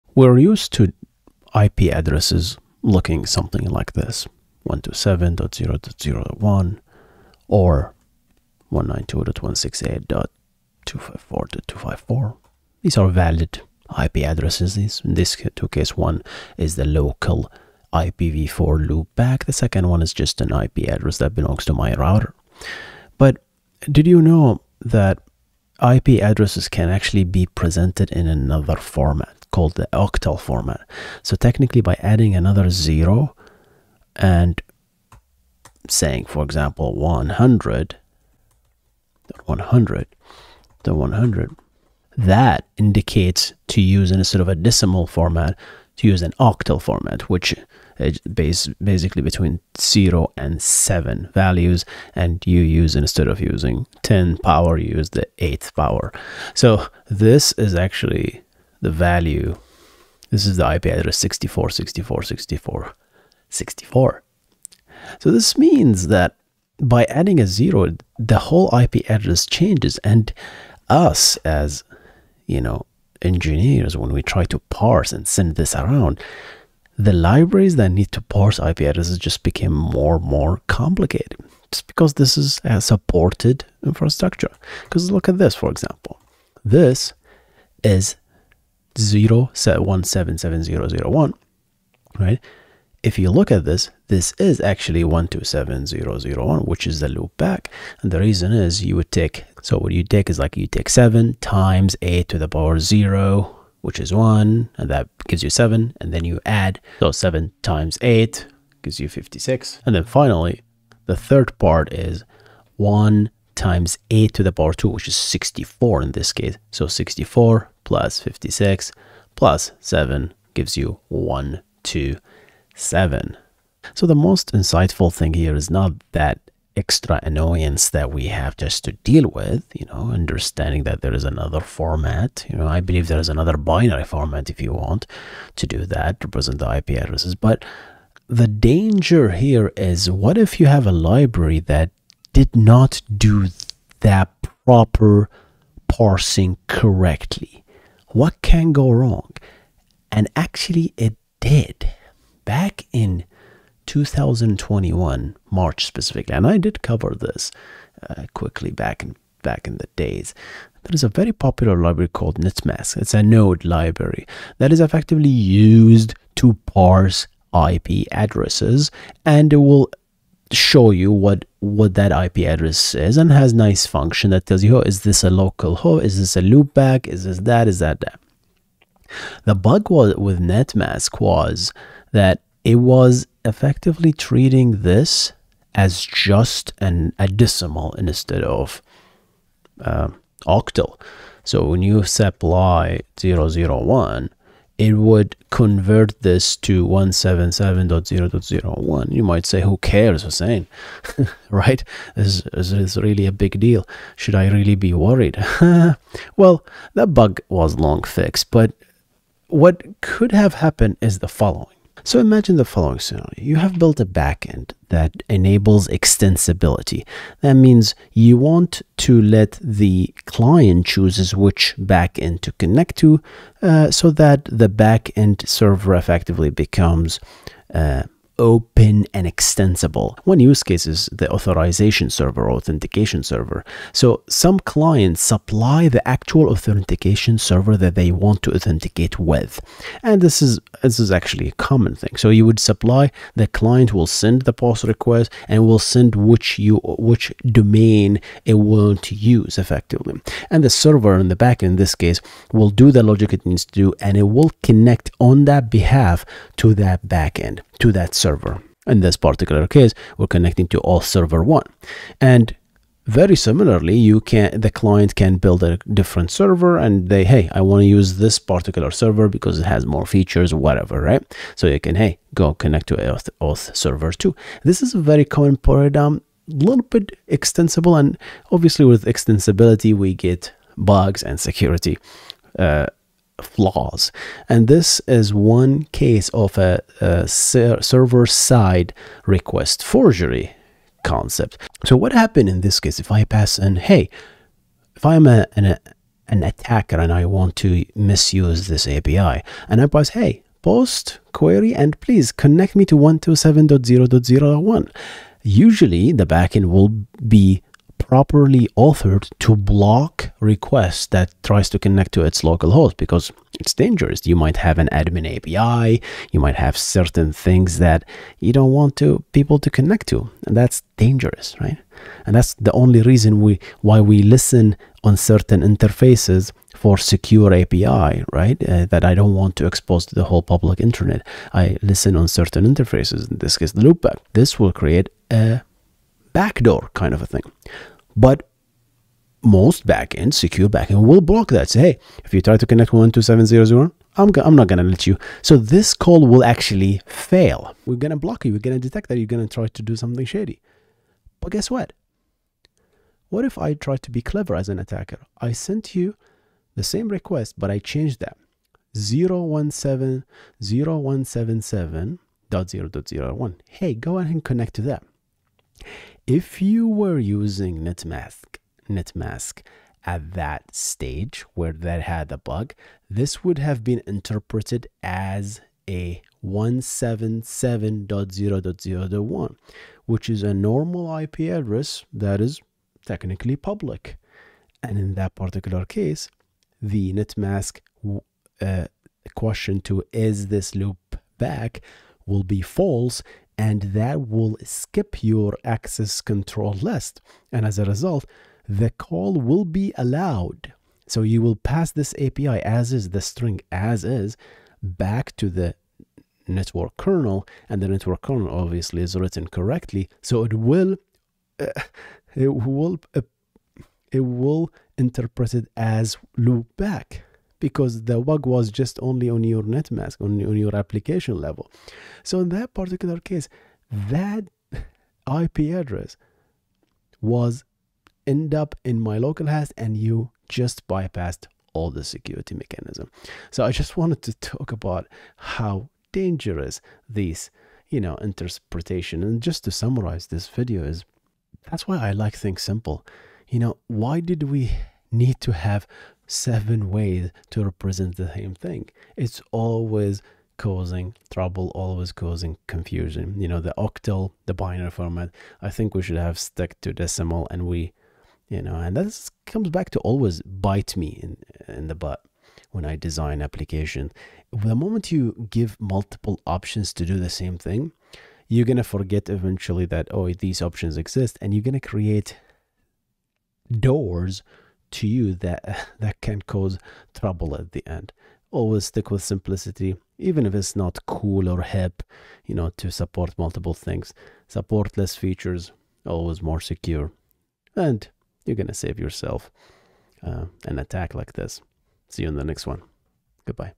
Why 0177.0.0.1 is also localhost
In this video I explain the octal format and the danger of incorrectly parsing IPv4 addresses and its effect on backend security.

Stay Awesome,
Hussein